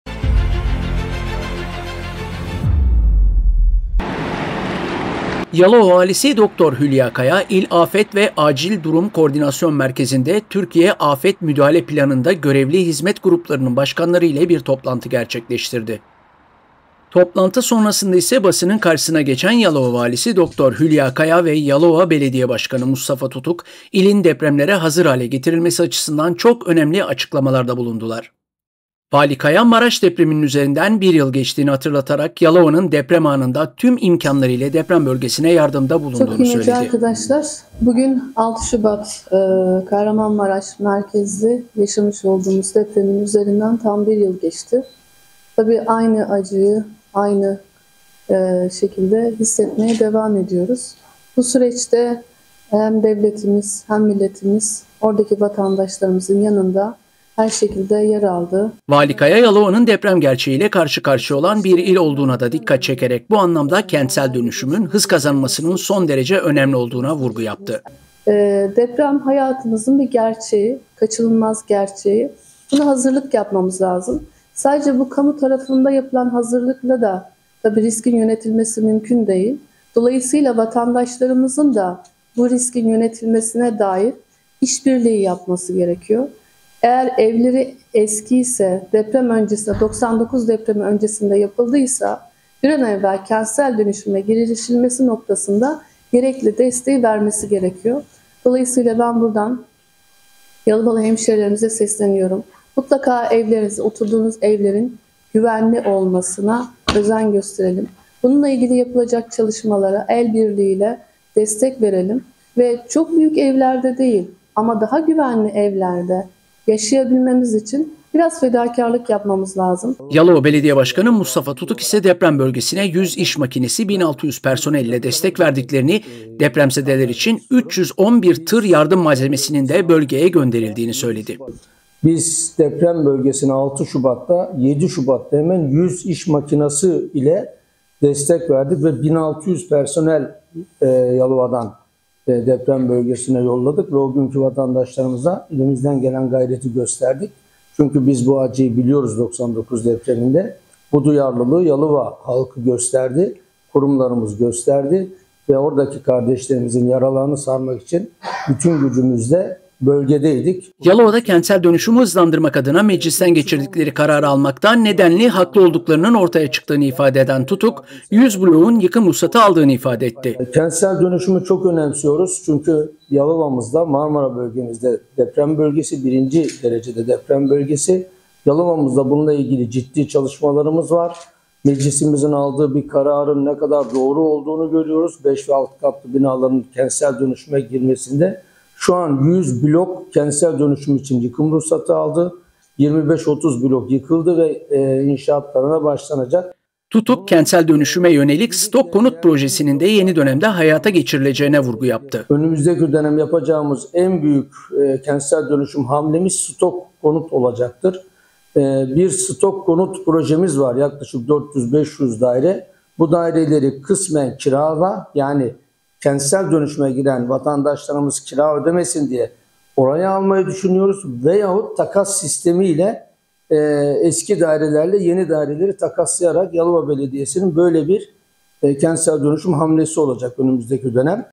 KAYA VE TUTUK’TAN, “KENTSEL DÖNÜŞÜM” VURGUSU
Yalova Valisi Dr. Hülya Kaya ve Yalova Belediye Başkanı Mustafa Tutuk Kahramanmaraş depreminin yıldönümü dolayısıyla düzenledikleri basın toplantısında ilde kentsel dönüşümün hız kazanmasının önemine vurgu yaptılar